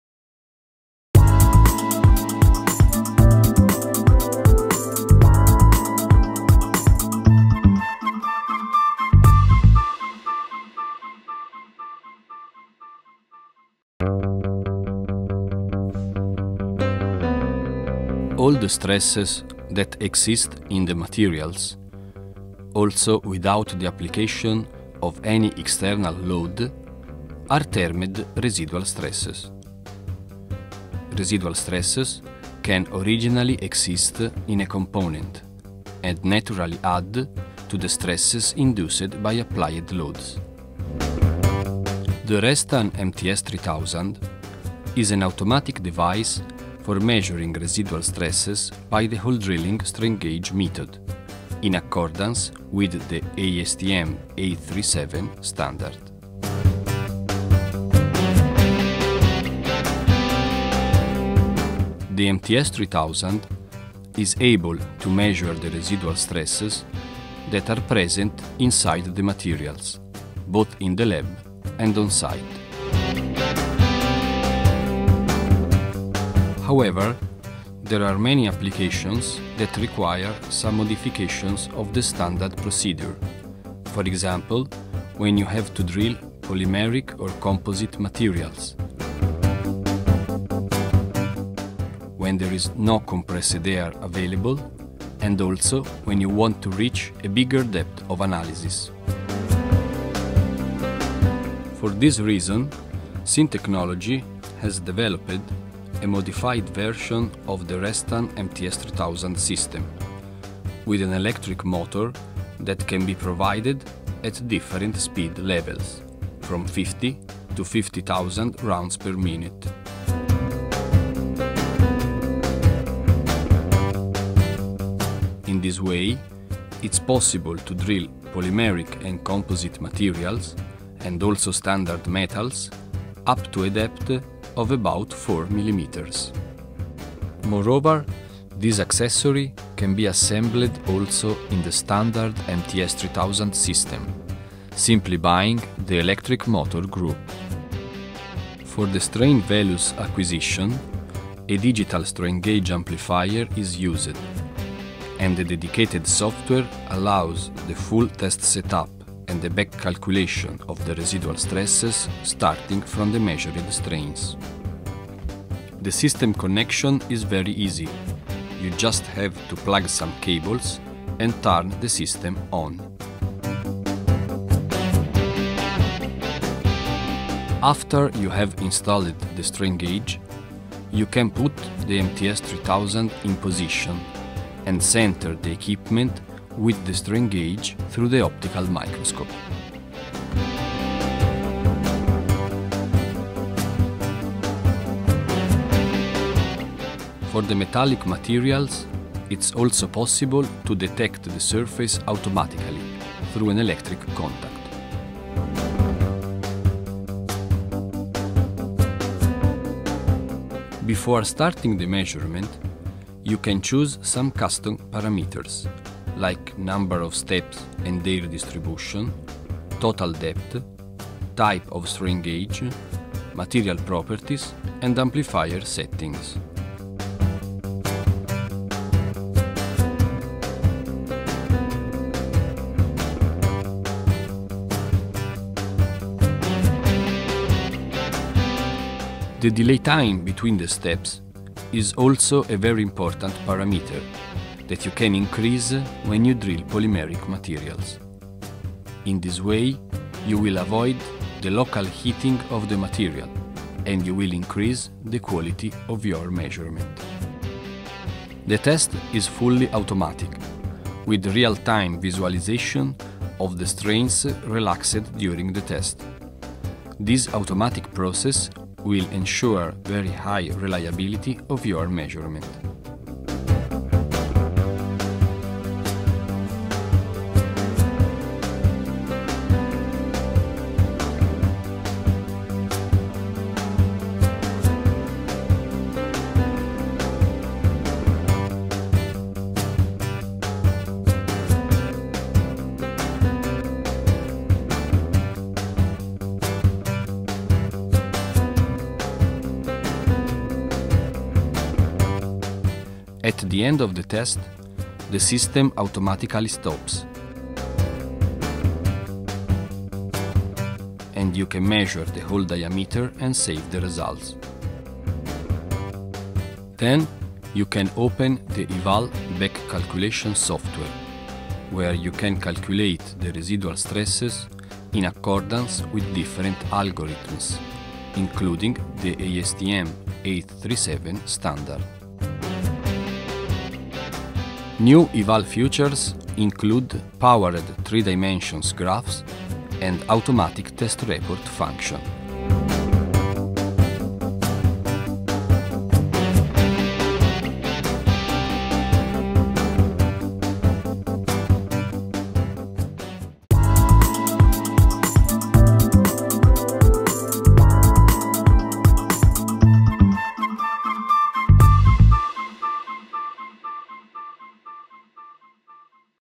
Residual Stress measurement - Restan MTS3000 - Electric motor version
The Restan MTS3000 – electric motor version is a modification of the standard system that allows to test metallic, composite and polymeric materials at different drilling speed. In this way there is no local melting of the component.
Moreover, this accessory is useful also in all the situations where there is no compressed air available.